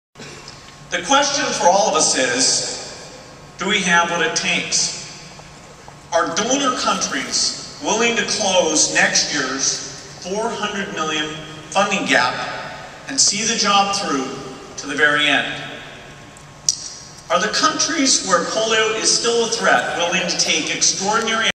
Bill Gates speaking to 20,000 fellow Rotarians
lucky Rotarians. Probably our most famous Fellow Rotarian spoke to us at the World Convention in New Orleans...and believe me there wasn't an empty seat in the house